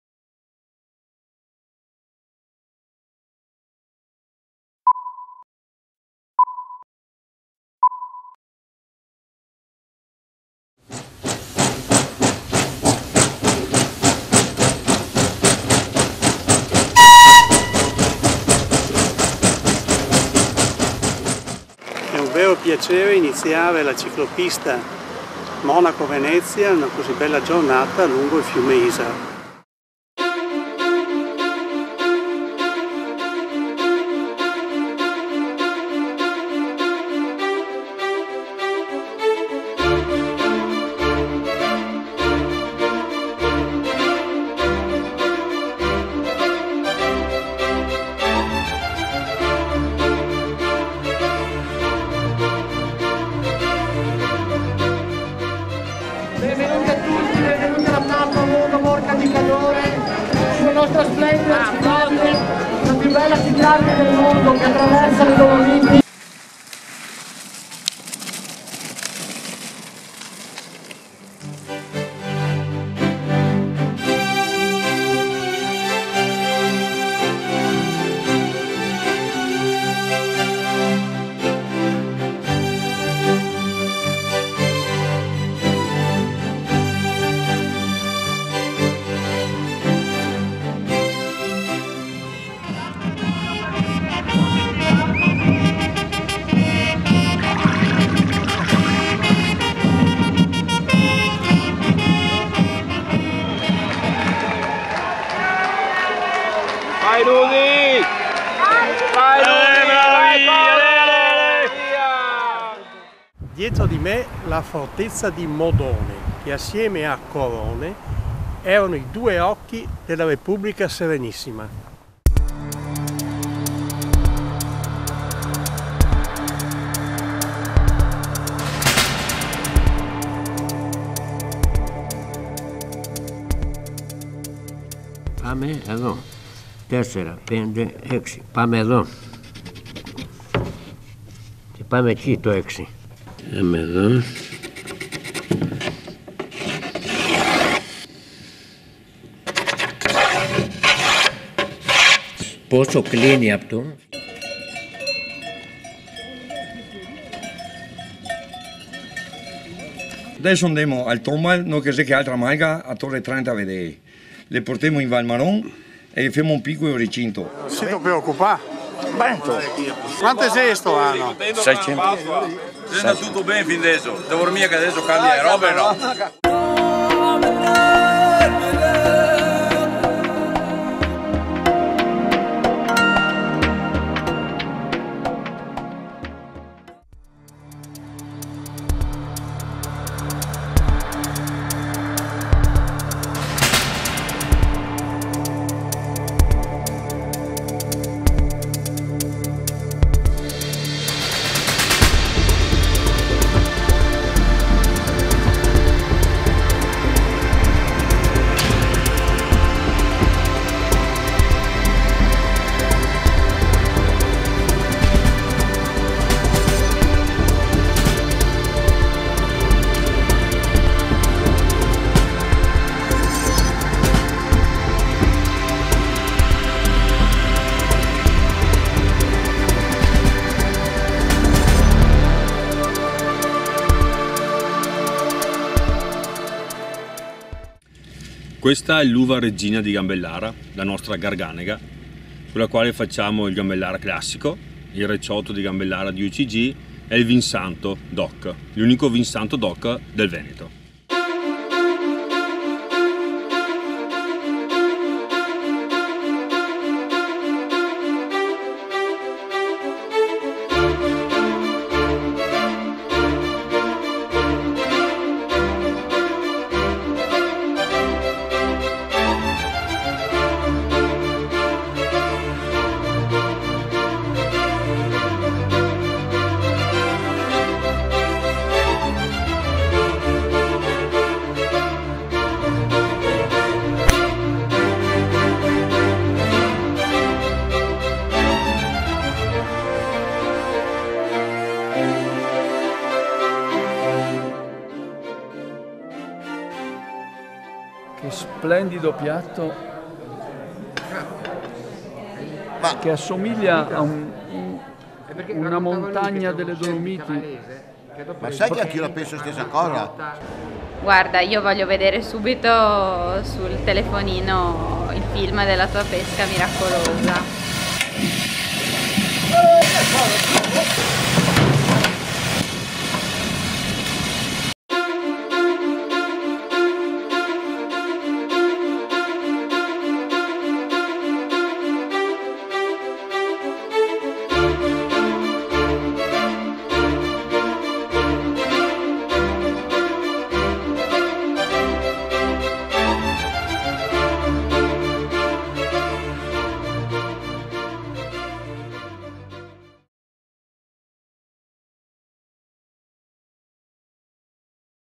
BelleEpoqueFilm Showreel
Showreel della Casa di Produzione Cinematografica BelleEpoqueFilm.it che festeggia nel 2019 mezzo secolo di passione e qualità.
Recapito: Biasioli Film - via Rinaldi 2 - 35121 Padova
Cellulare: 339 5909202